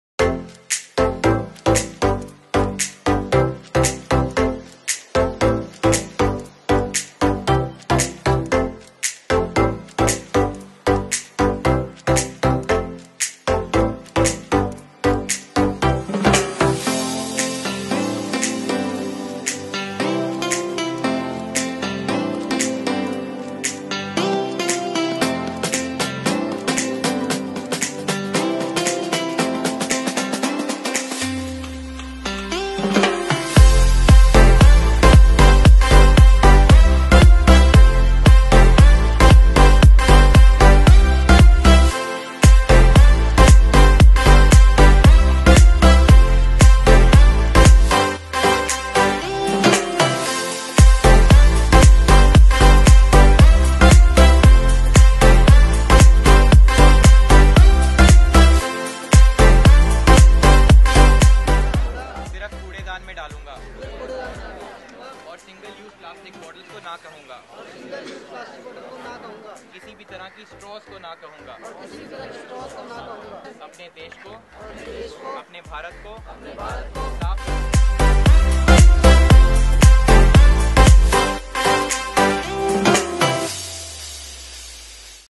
Felicitation of Ripu Daman Bevli as the FIT India Ambassador
Also known as the Plogman of India, Ripu Daman Bevli credited with creating India's first eco-fitness movement combining running and fitness to fight the garbage crisis in the country. On December 2019, he was conferred the title of the FIT India Ambassador by the Honorable Sports Minister of India.